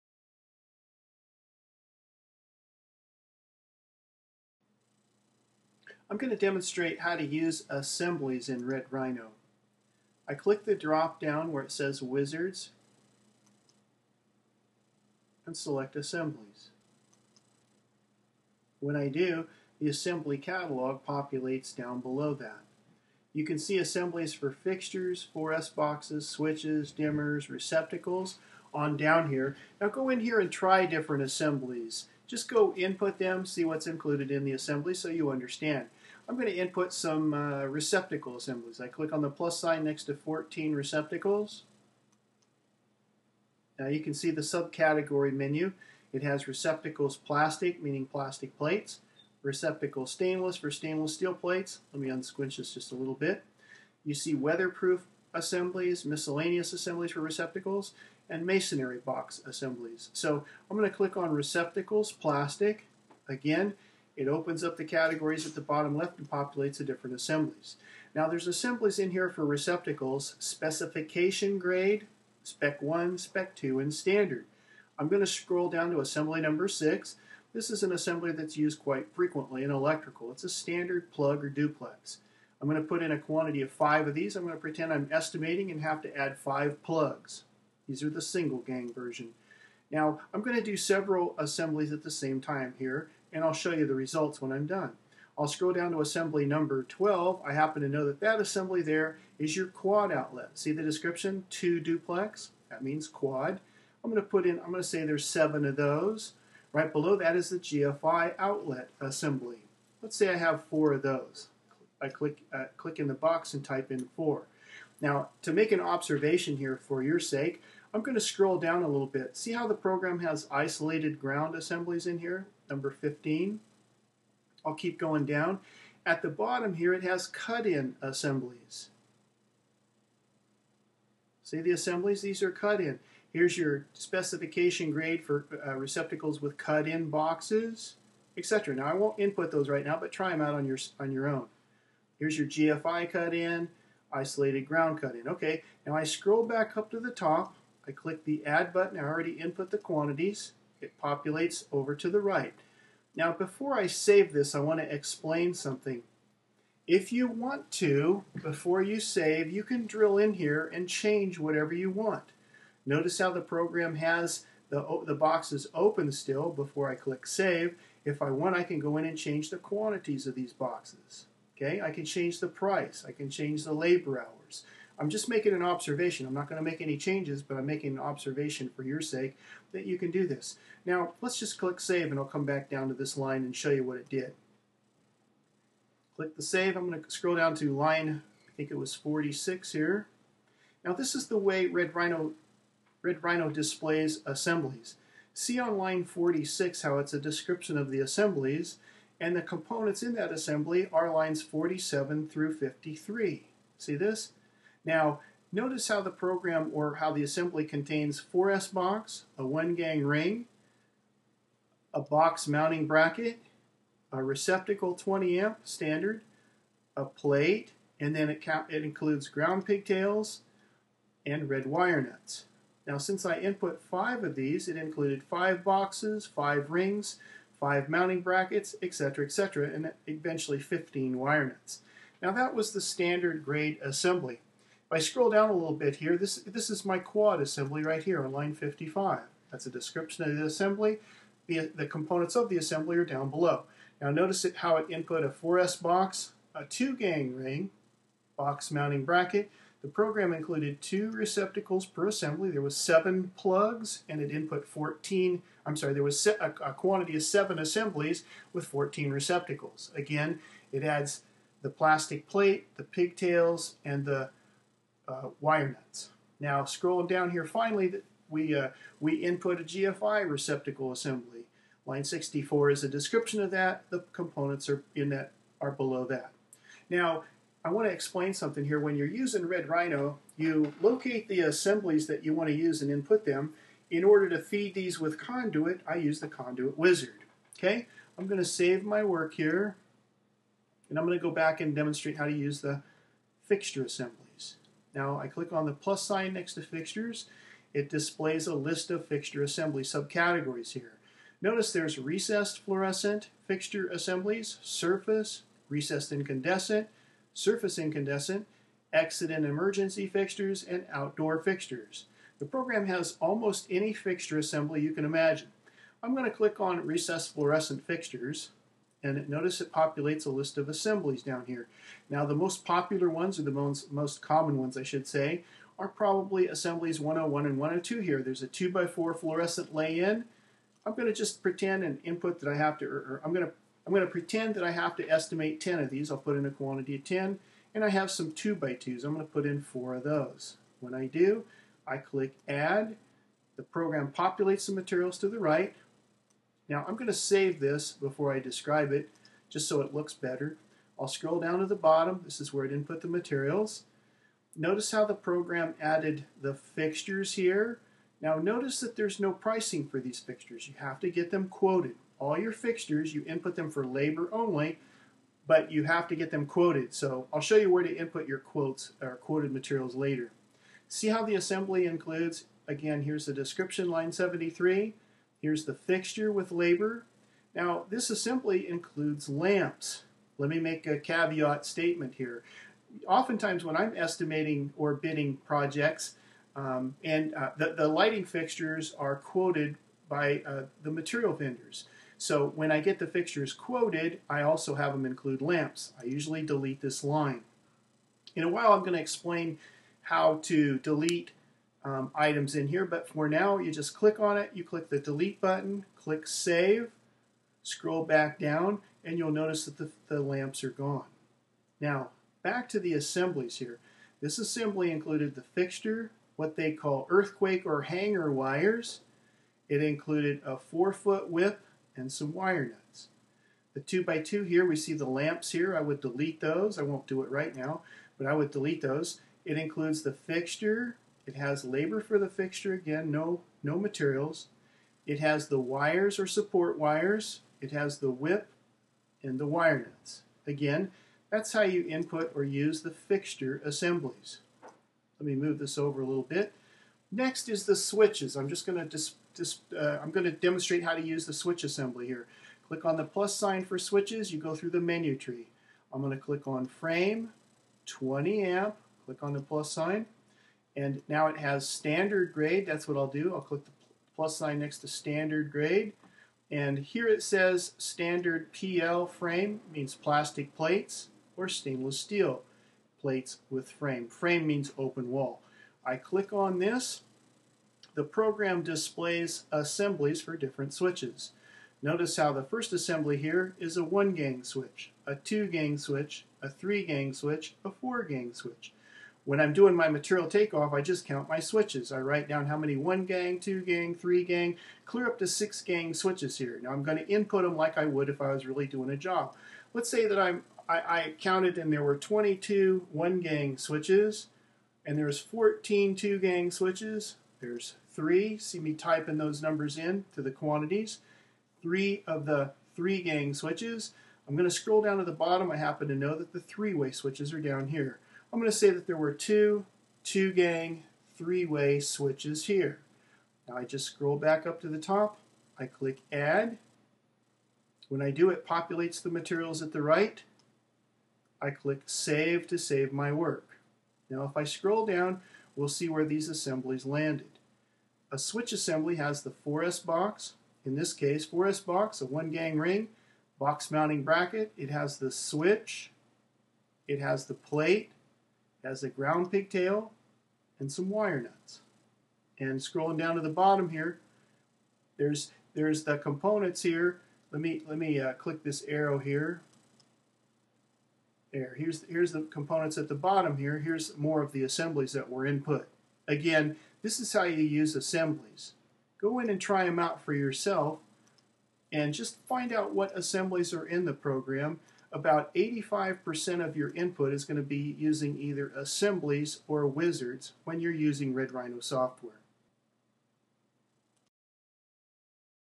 Electrical Estimating, Estimating Electrical Work
Electrical Estimating, Estimating Electrical Work is fast using assemblies. Assemblies are lists of material needed to install items like plugs, light fixtures and switches. Red Rhino Electrical Estimating Software provides you with hundreds of pre-built assemblies for rapid input of materials.